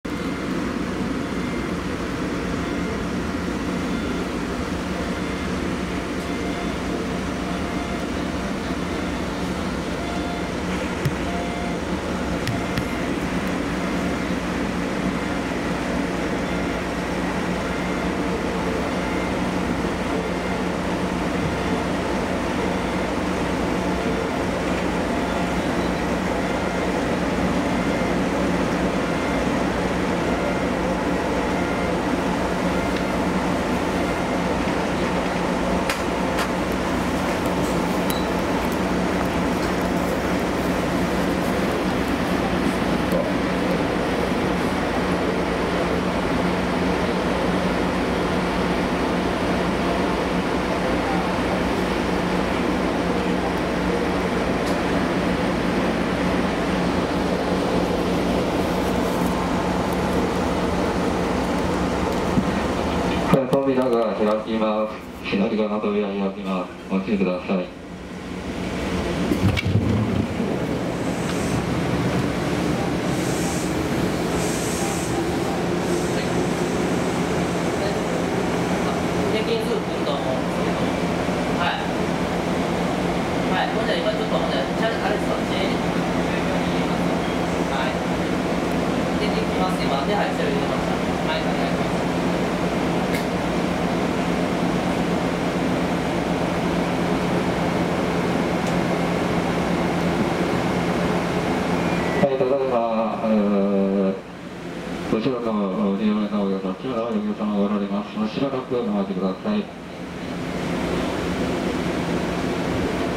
車内非常ボタン取り扱いあり！
今宮駅到着時に車内非常ボタンが取り扱われたため、扉が開くまで若干時間がかかりました。急病の人がいたようです。